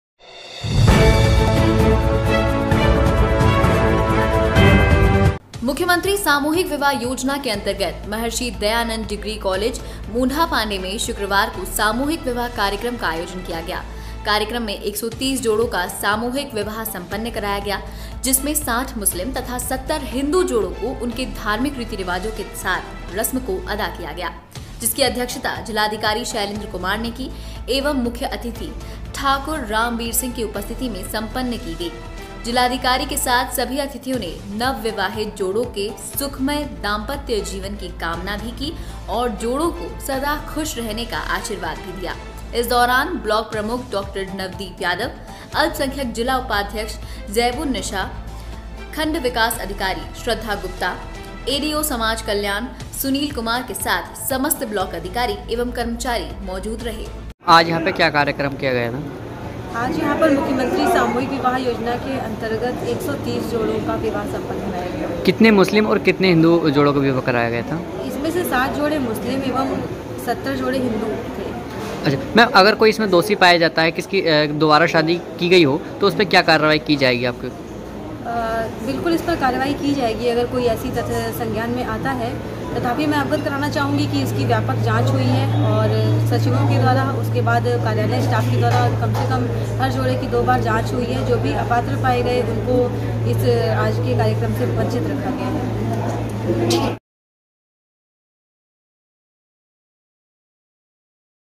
मुरादाबाद..मुख्यमंत्री सामूहिक विवाह में 130 जोड़े एक-दूसरे के हुए, जिसमे 60 मुस्लिम व 70 हिंदू जोड़े
मूंढापांडे, मुरादाबाद.. मुख्यमंत्री सामूहिक विवाह में 130 जोड़े एक दूसरे के हुए

मूंढापांडे: मुख्यमंत्री सामूहिक विवाह योजना के अन्तर्गत महर्षि दयानंद डिग्री कॉलेज मूंढापाण्डे में शुक्रबार को सामूहिक विवाह कार्यक्रम का आयोजन किया गया। कार्यक्रम में 130 जोड़ों का सामूहिक विवाह सम्पन्न कराया गया। जिसमें 60 मुस्लिम तथा 70 हिंदू जोड़ों को उनके धार्मिक रीति-रिवाजों के साथ रस्म को अदा किया गया। जिसकी अध्यक्षता जिलाधिकारी शैलेन्द्र कुमार ने की एवं मुख्य अतिथि ठाकुर रामबीर सिंह की उपस्थिति में संपन्न की गई।जिलाधिकारी के साथ सभी अतिथियों ने नवविवाहित जोड़ों के सुखमय दाम्पत्य जीवन की कामना की और जोड़ो को सदा खुश रहने का आशीर्वाद दिया। इस दौरान ब्लॉक प्रमुख डॉ नवदीप यादव, अल्प संख्यक ज़िला उपाध्यक्ष जैबुन निशा, खंड विकास अधिकारी श्रद्धा गुप्ता , एडीओ समाज कल्याण सुनील कुमार के साथ समस्त ब्लॉक अधिकारी एवं कर्मचारी मौजूद रहे।

वाइट:- विकास खंड अधिकारी श्रद्धा गुप्ता

संवाददाता :- फिरोज खान